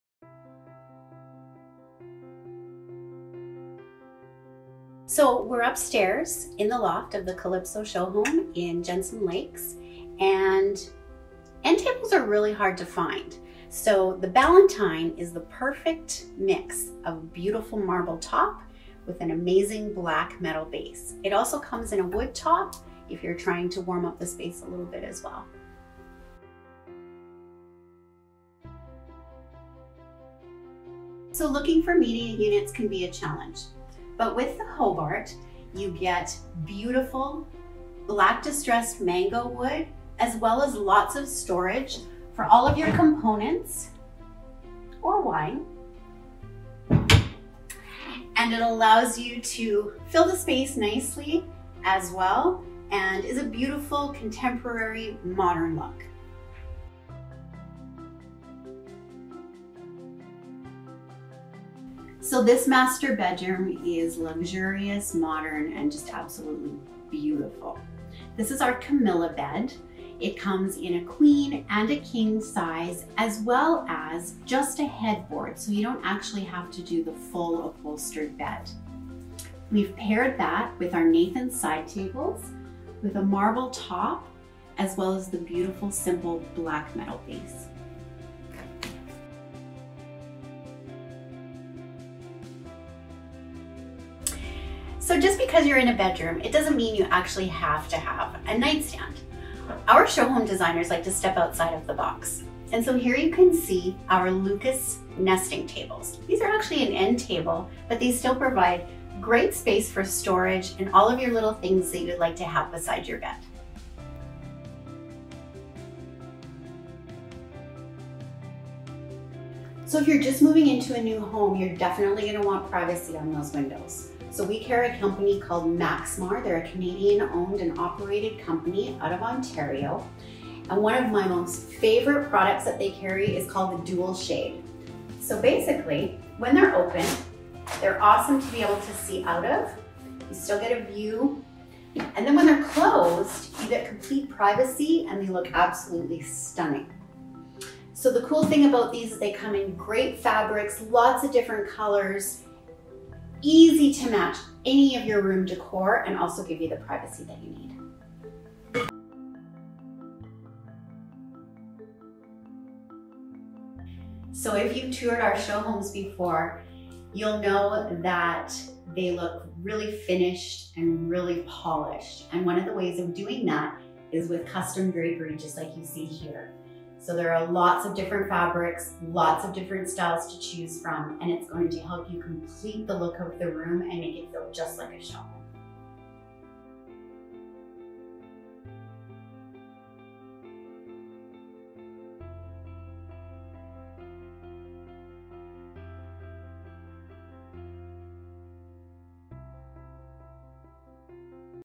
Furniture Tour - Calypso in Jensen Lakes - Upper Floor
Join us on a virtual tour of the Jensen Lakes Calypso showhome by Pacesetter Homes. We will take you through all of the furniture selections available at designQ Shop. Interested in owning one of these gorgeous pieces?